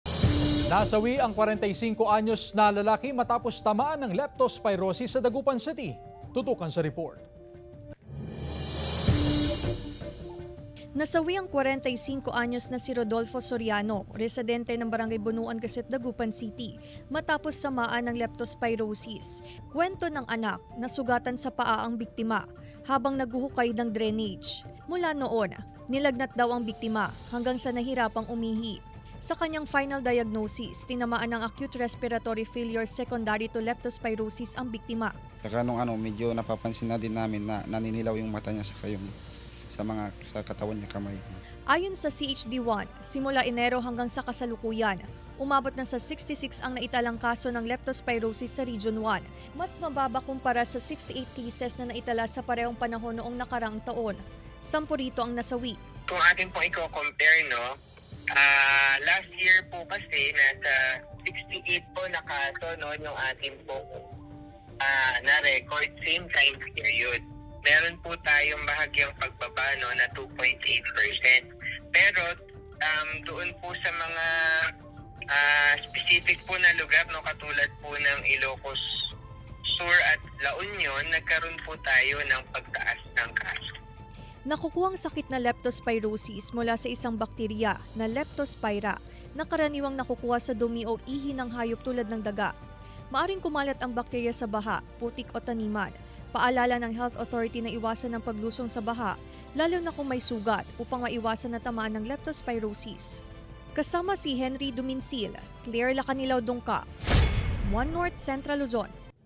One North Central Luzon: 66 na kaso ng leptospirosis, naitala ng CHD-1 mula January-August 2024
66 na kaso ng leptospirosis, naitala ng CHD-1 mula January-August 2024.